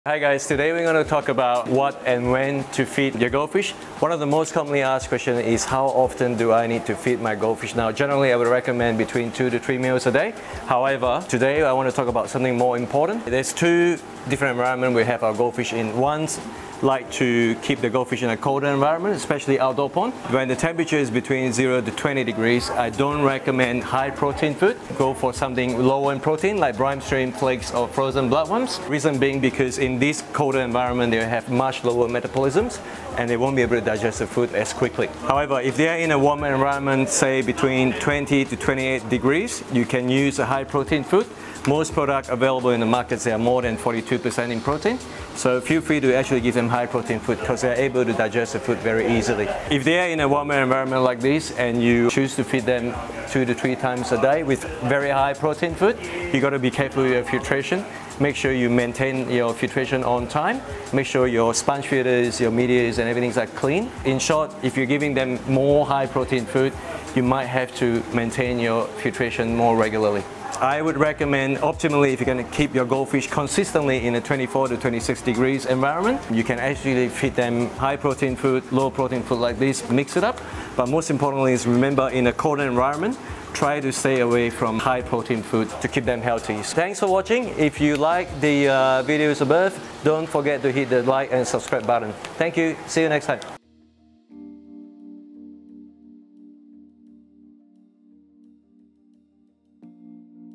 Is Your Goldfish Digesting Protein? | Low vs high protein, ideal water temperature
A big realisation we had at OZ Discus was goldfish don't always absorb protein very well.

If you have the temperature cold your goldfish will have issues with digesting protein, and the food the goldfish is eating may be bad for the gold fishes digestion.

Vincent explains all the important things you should know when feeding your goldfish also.

How many times a day should you feed your goldfish?
What should you feed your goldfish?
The importance of water temperature when feeding your goldfish.

Or get in touch with Vincent at OZ Discus.